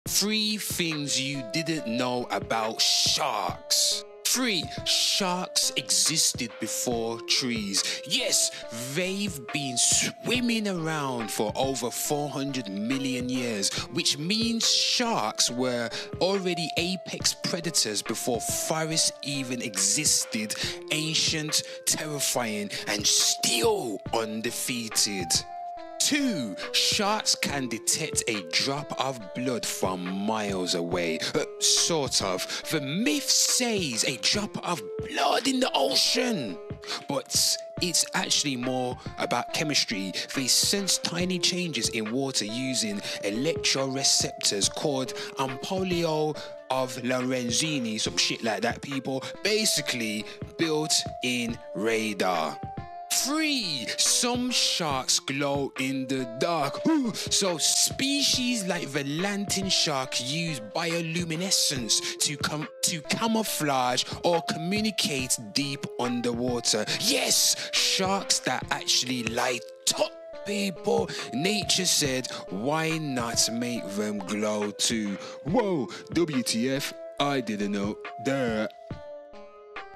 3 Things You Didn't Know About Sharks!
Sharks are older than trees, can sense tiny electric fields, and some even glow in the dark. They’ve survived every mass extinction and still rule the deep. Basically — ancient, electric, and bioluminescent killing machines. 🦈⚡💡